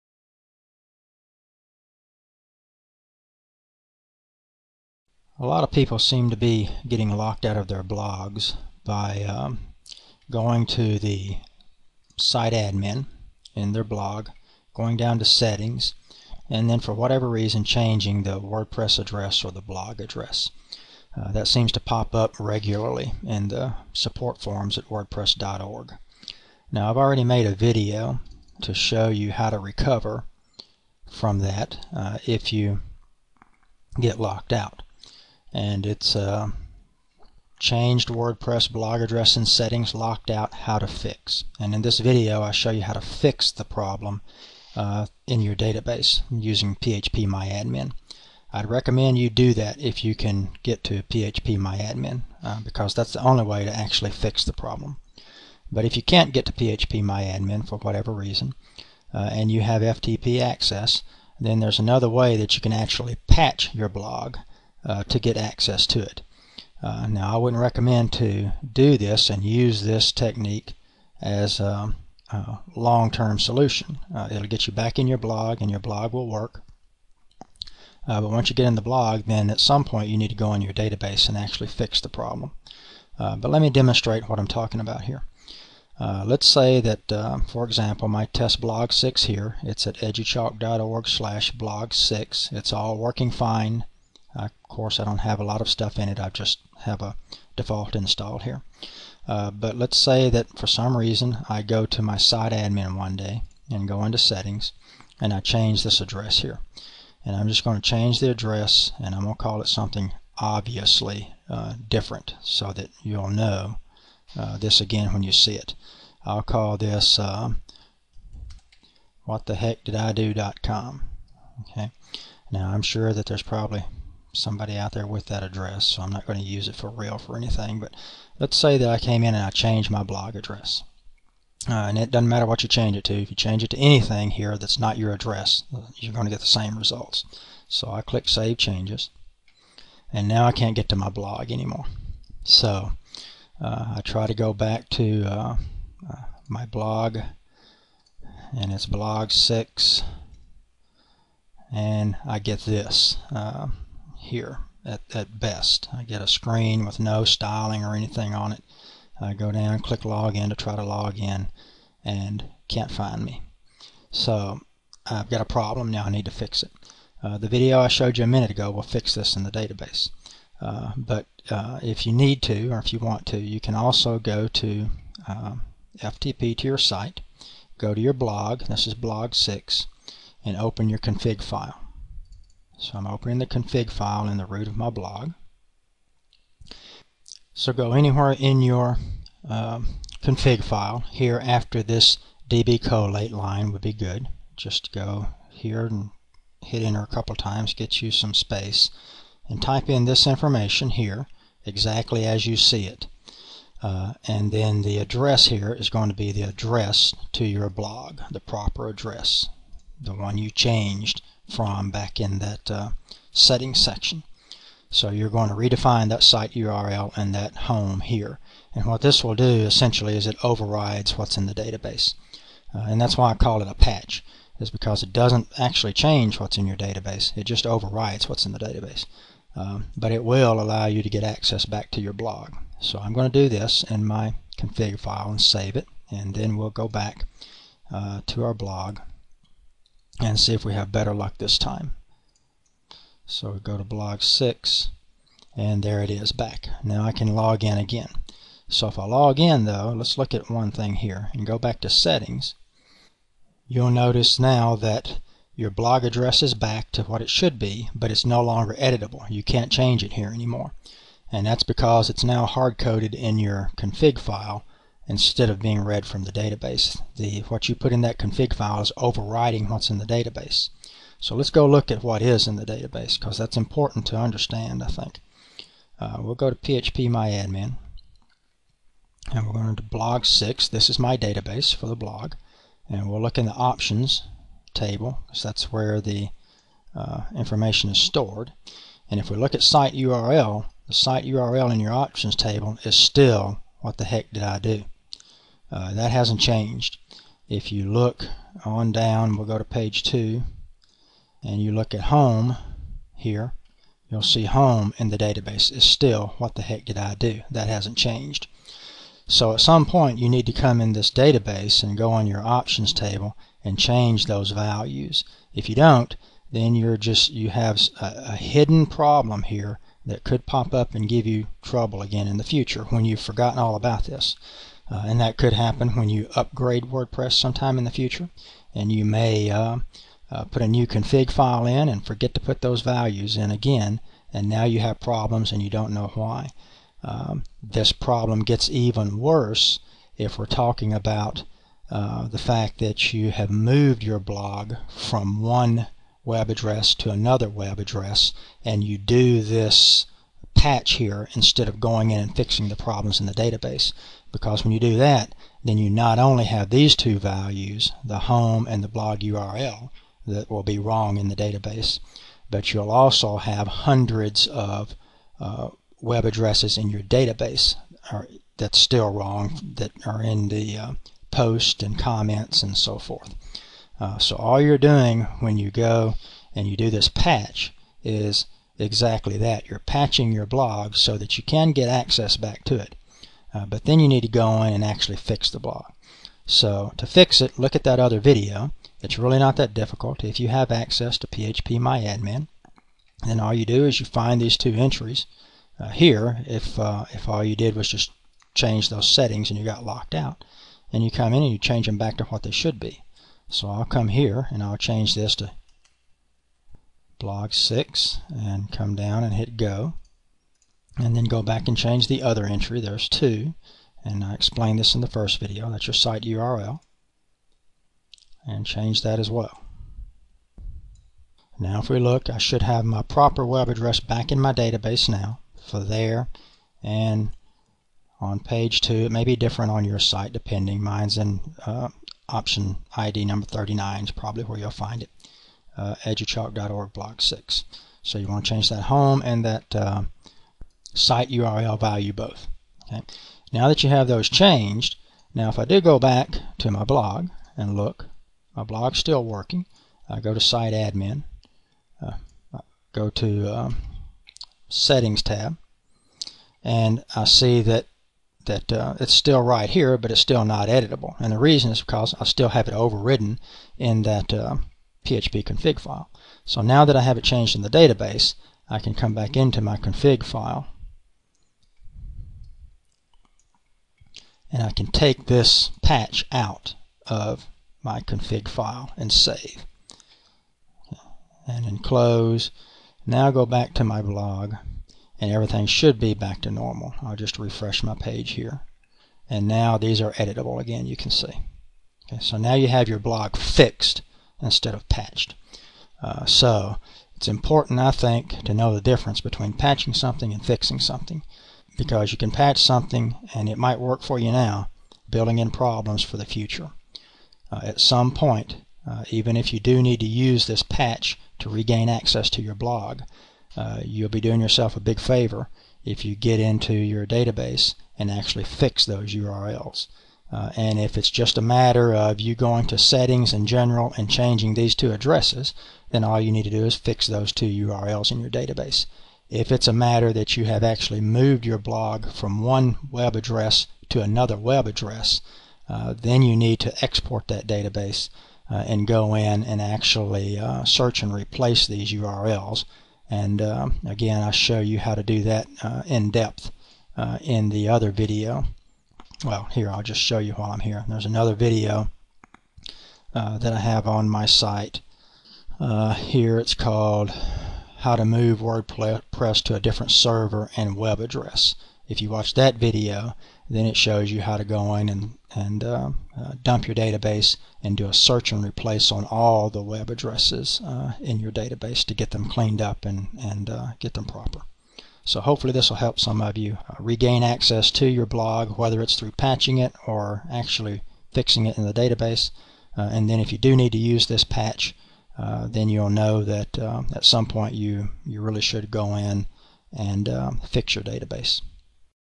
Fixing vs Patching Your WordPress Blog: What You Should Know
Changed Your Blog URL and Broke It? You Can Patch it, or Fix it -- I recommend fixing it.